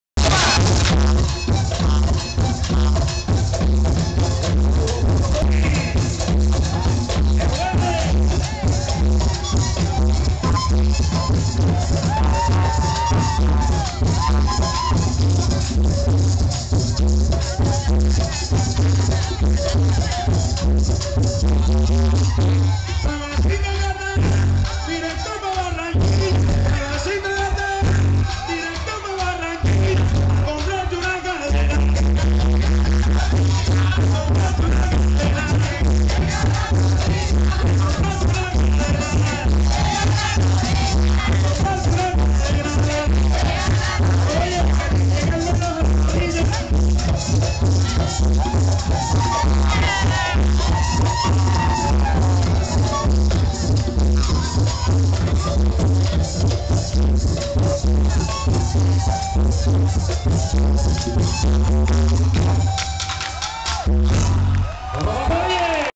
ANIVERSARIO DE COLOMBIA 2017 EN BUENOS AIRES
FIESTA DE LA COLECTIVIDAD COLOMBIANA EN AVENIDA DE MAYO